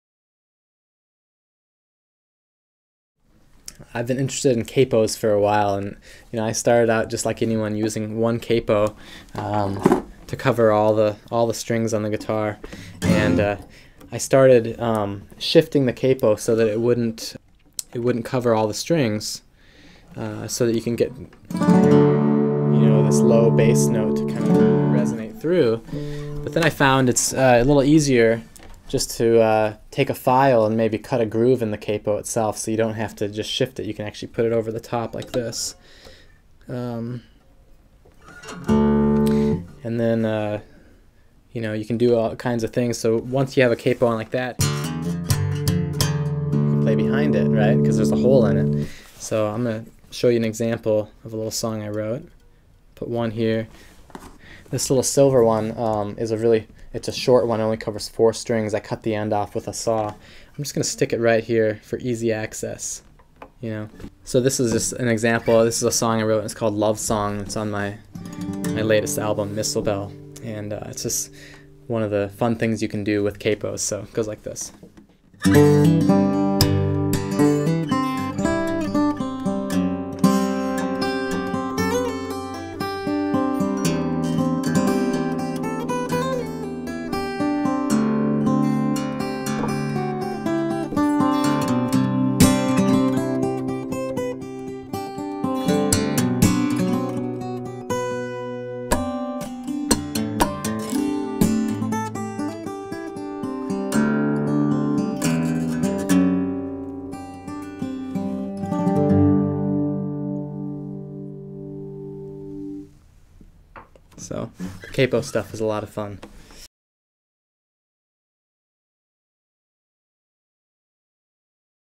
Acoustip June 2010 - Trace Bundy
Watch as McPherson Artist Trace Bundy shows a tip/trick he has picked up over the years.  For the latest Acoustips, become our friend